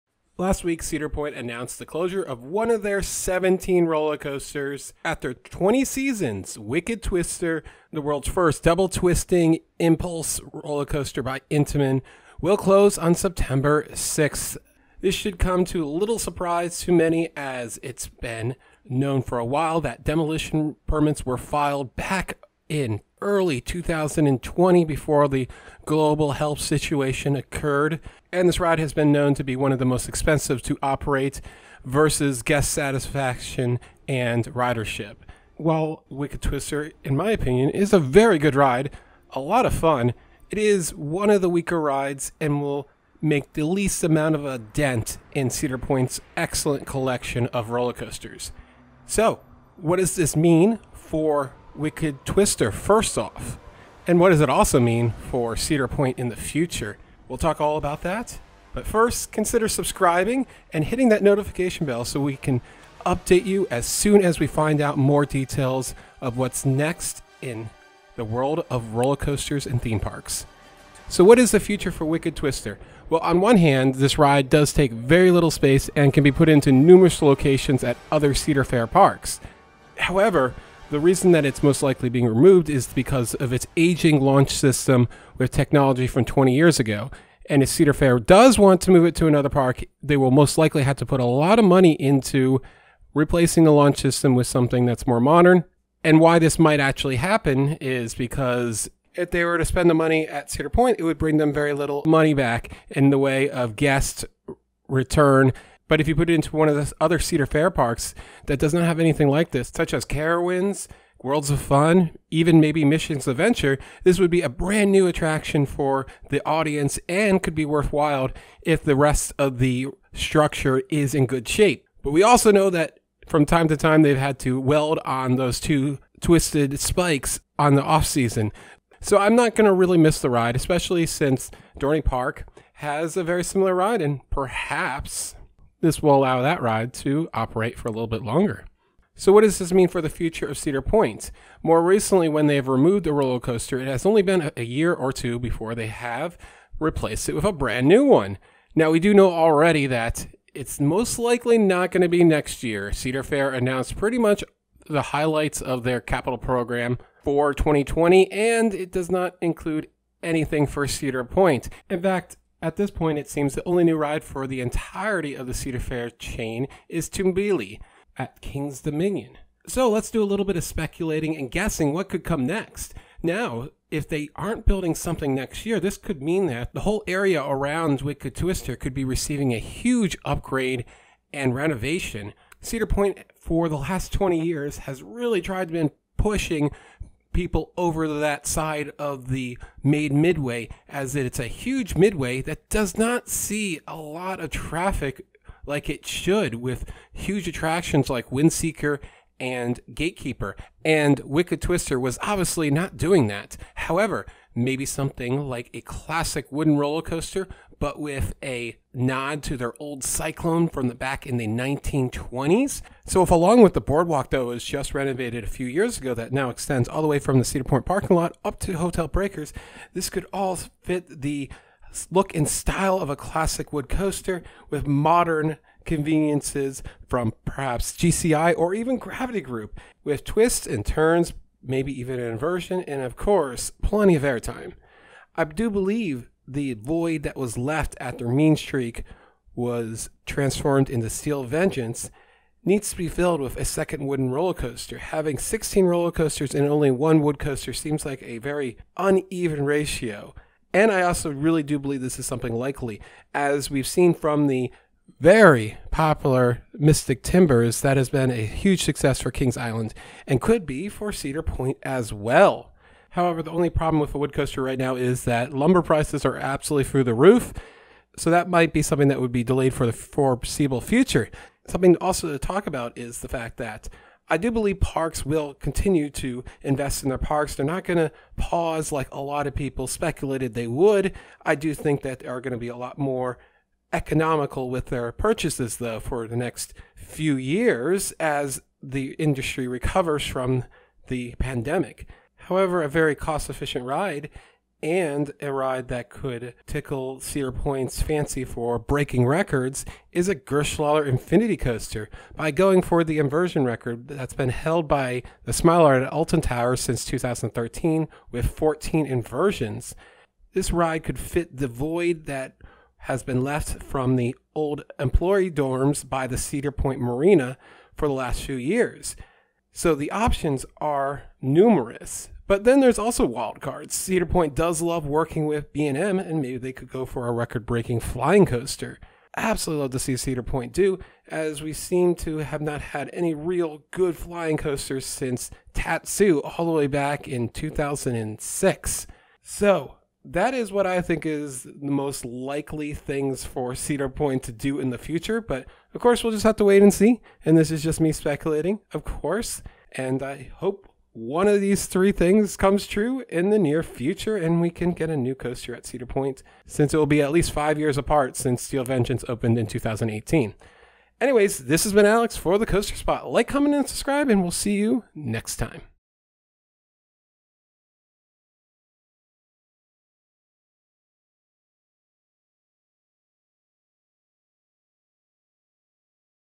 Wicked Twister is Closing, What's Next for Cedar Point | News | August 2021
Cedar Point has announced that Wicked Twister will close on September 6th. Alex discusses the future for Wicked Twister and Cedar Point.